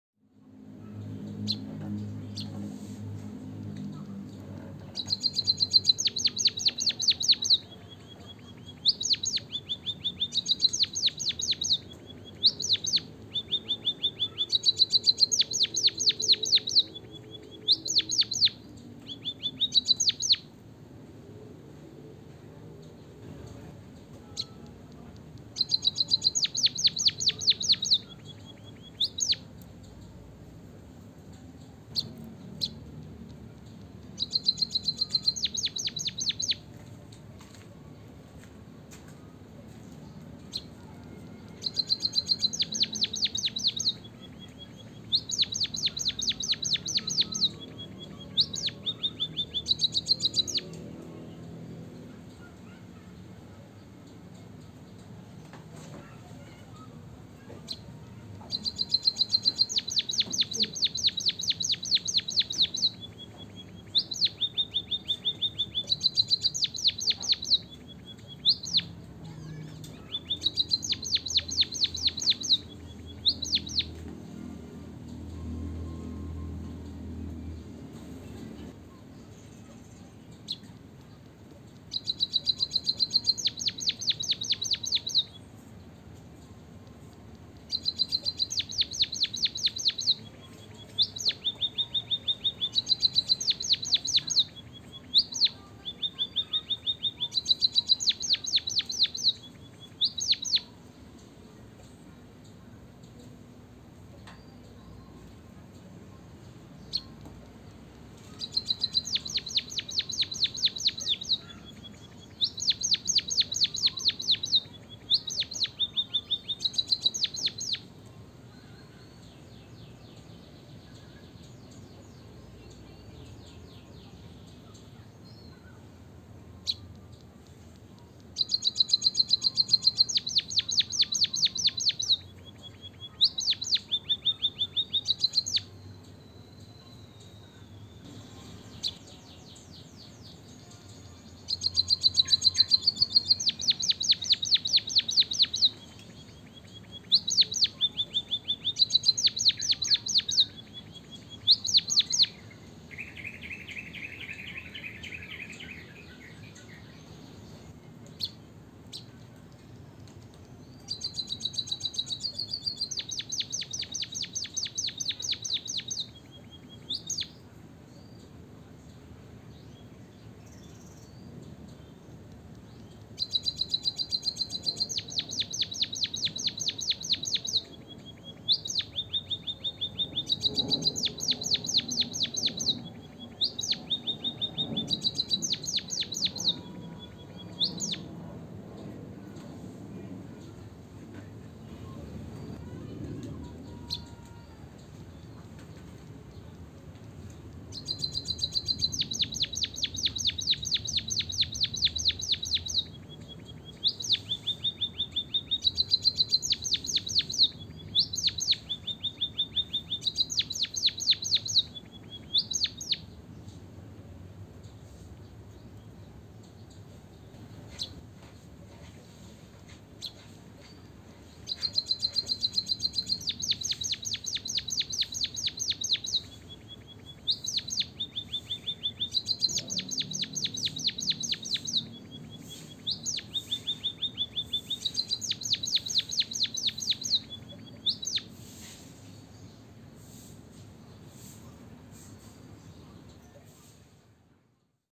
emprit kaji gacor - suara bunyi burung pipit untuk pancingan
emprit kaji gacor - suara bunyi burung pipit untuk pancingan emprit yang malas bunyi.